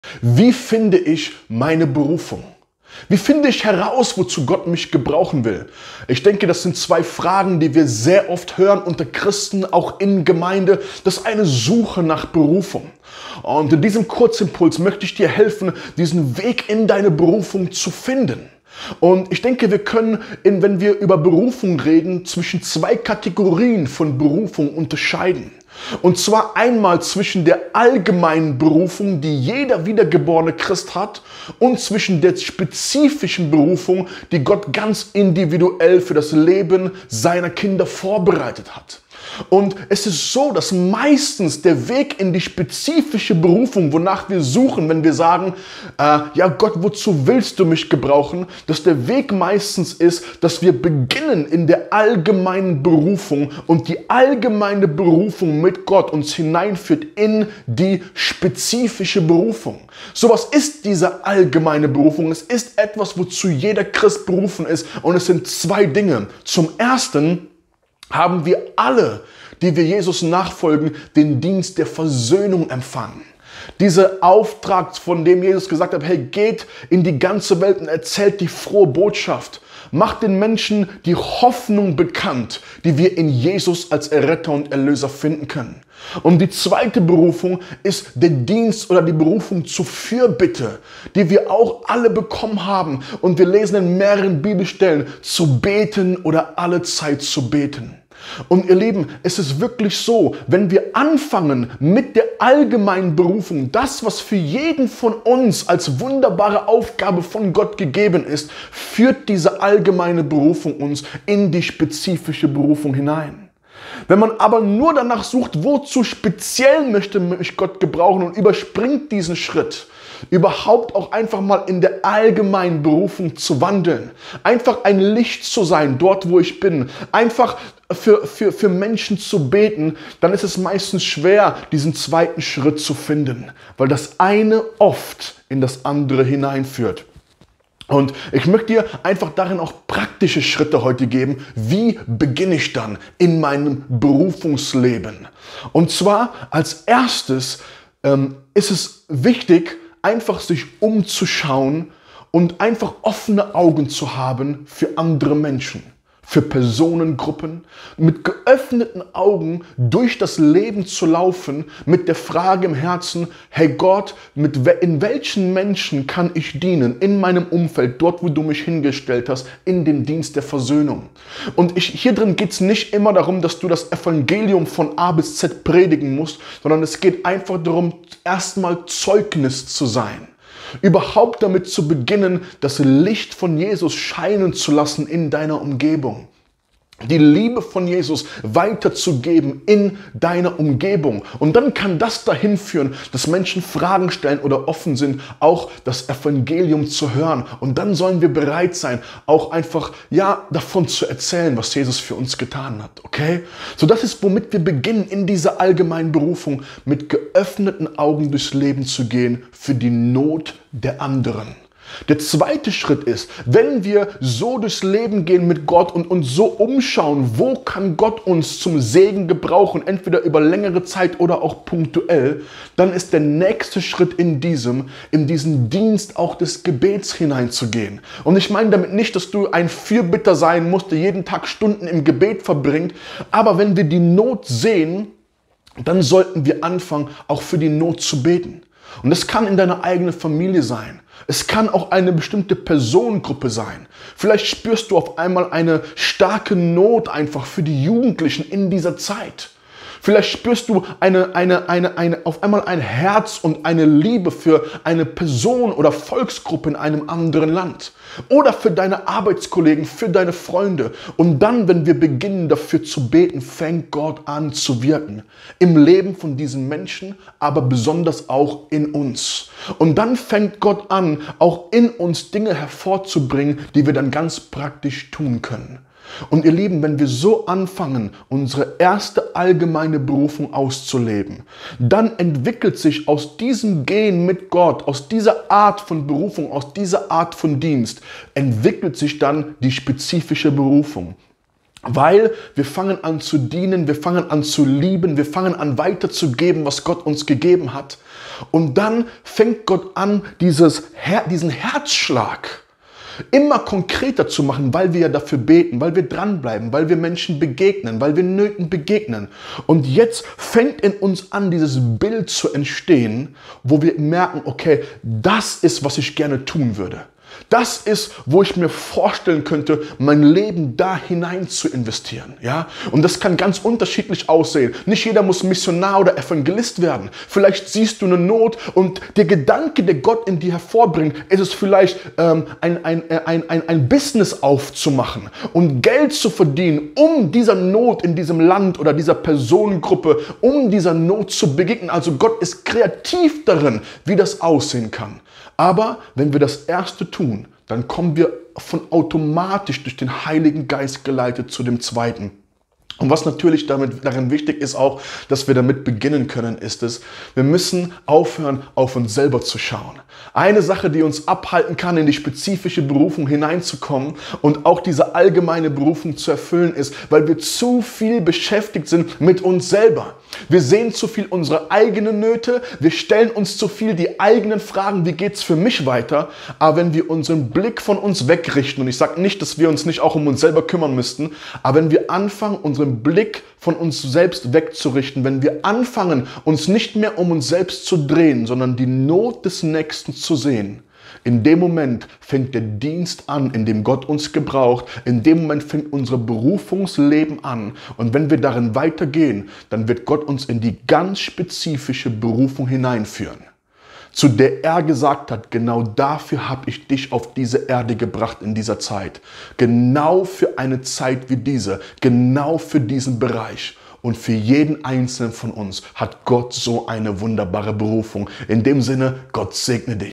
Wie du deine Berufung von Gott finden kannst | Kurzimpuls | Nathanael Aclassato
Finde deine Berufung - Der Kurzimpuls

| Ermutigen von Christen in der Nachfolge
| Stärkung im Glauben
| Auferbauung in Herausforderungen, Krisen & Prüfungen